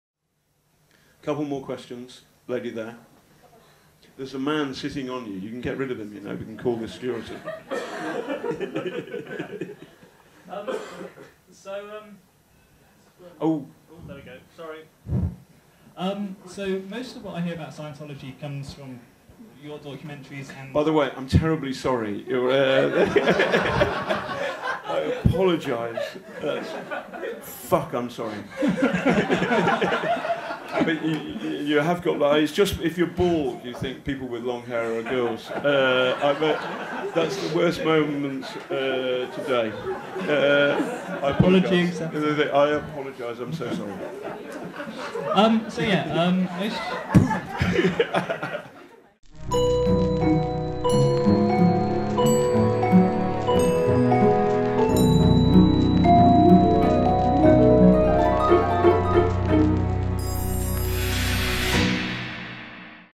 John Sweeney: Scientology sceptics
John using his journalist & detective skills (apologies for his bad language).